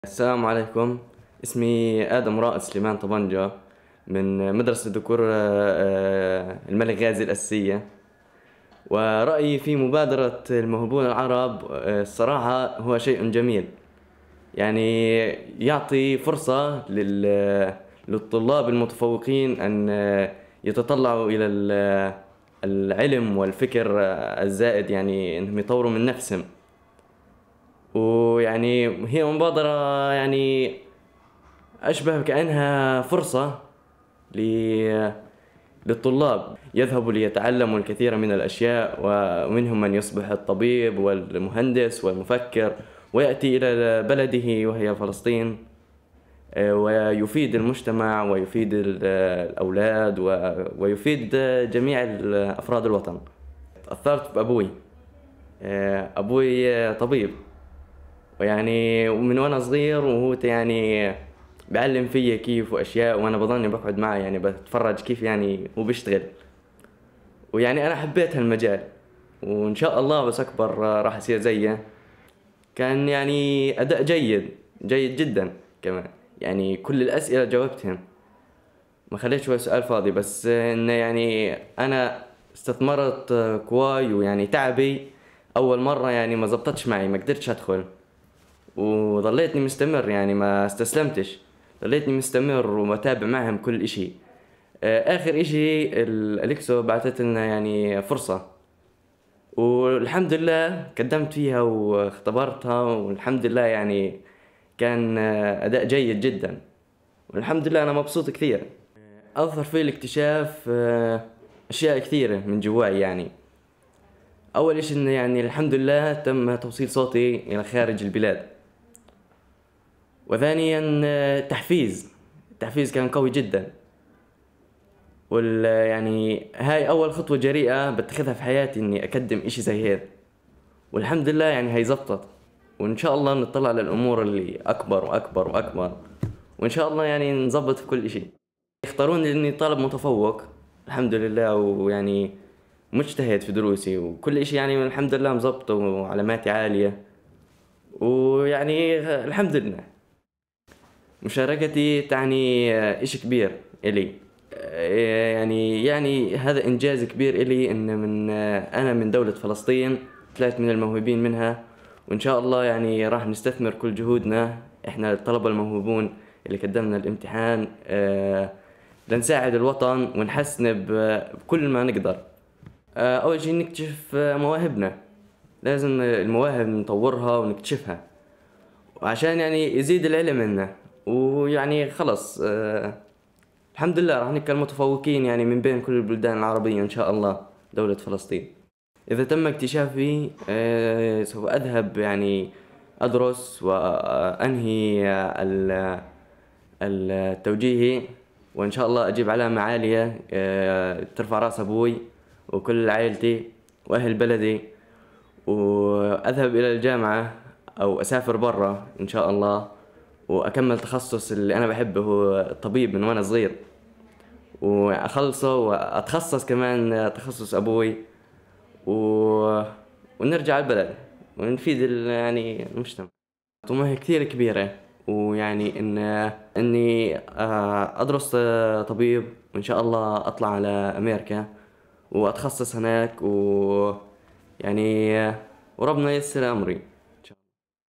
رائد طبنجة يروي قصته مع
من أرض فلسطين الأبية..

يحكي لنا الطالب الموهوب رائد طبنجة عن تجربته في مبادرة الموهوبون_العرب : "هي فرصة للطلاب المتفوقين بأن يتطلعوا إلى اكتساب العالم والمعرفة ليفيدوا مجتمعاتهم".